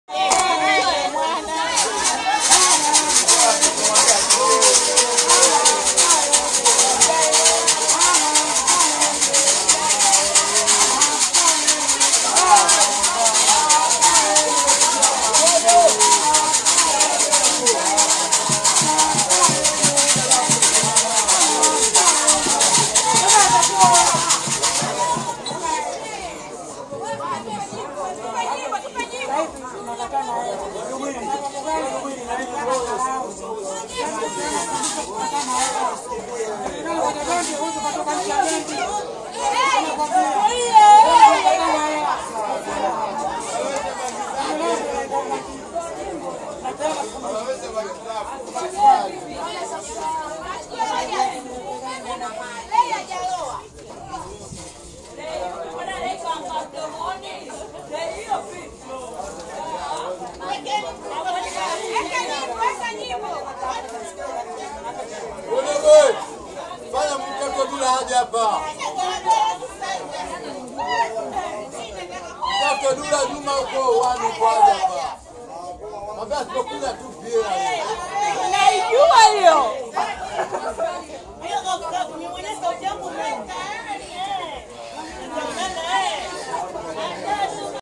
CHURA WA TANDALE WAVUA-NGUO PAMBE NDANI Ya MSASANI : CHURA KAMA WOTE, SHUGHULI Za USWAHILINI USIKU
MSASANI KIGODORO USIKU BALAAA : CHURA KAMA WOTE, TAZAMA HUONE HATARI TUPU!